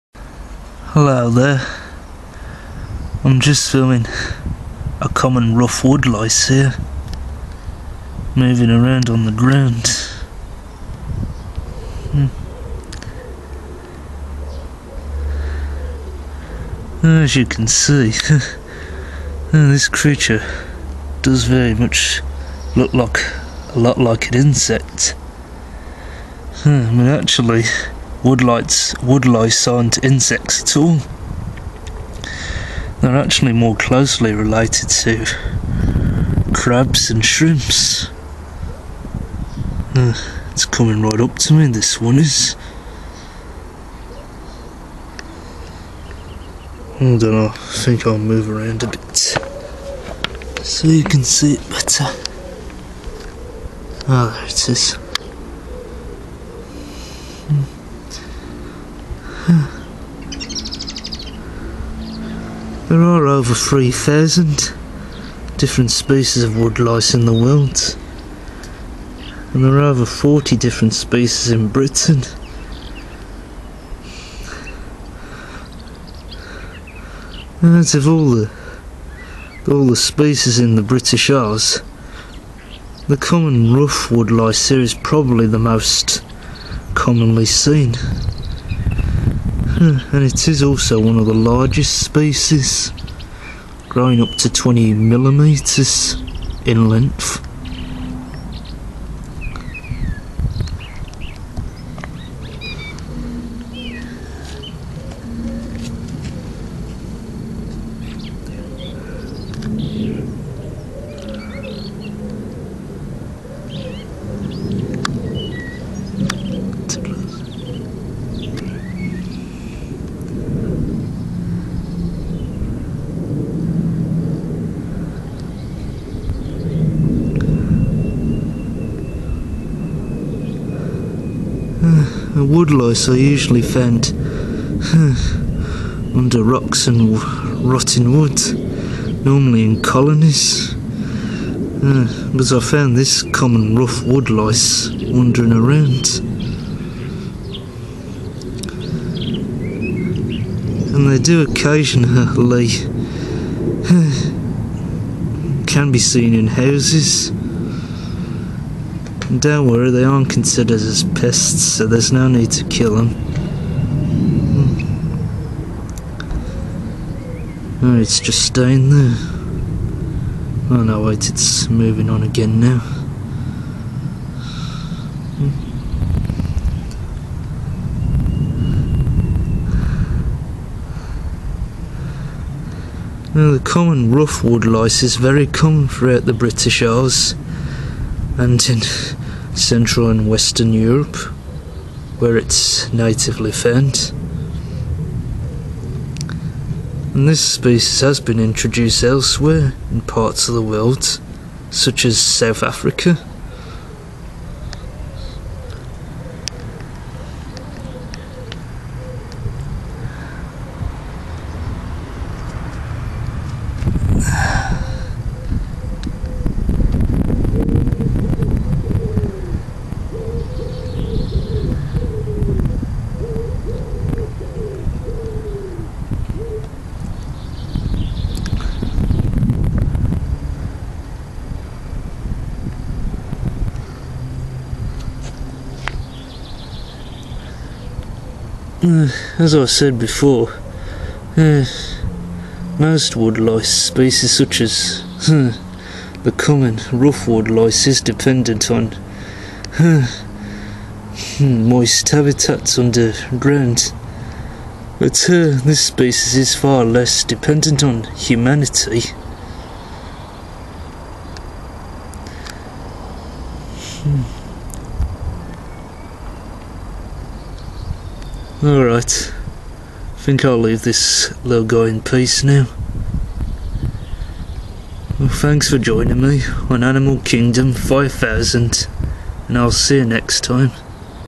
A.K.5000 Common Rough Woodlouse
This is the common rough woodlouse on my channel now guys. It is one of more than 3,000 species on the planet. The common rough woodlouse lives in wide variety of habitats which contain reasonable levels of humidity, but it is far less depentdent on water than the common shiny woodlouse (Oniscus asellus). It is probably the most commonest woodlouse found in the United Kingdom, sometimes even showing up in houses where it and many other species can die from dehydration. The common rough woodlouse is usually grey in colour, but can be mottled with yellow or brown. It usually gets its name from the rough upper surface on its back. I filmed this common rough woodlouse wandering around in my garden.

Other Facts

Kingdom: Animalia
Phylum: Arthropoda
Subphylum: Crustacea
Class: Malacostraca
Order: Isopoda
Suborder: Oniscidea
Family: Porcellionidae
Scientific Name: Porcellio scaber
Length: Up to 20 mm
Distribution: Originally from Central & Western Europe; introduced into North America, South Africa & Australia
Conservation Status: Not Evaluated

All facts and information are based on what I've researched on the internet or read about in books or heard from other animal experts when I travel to other different countries around the world.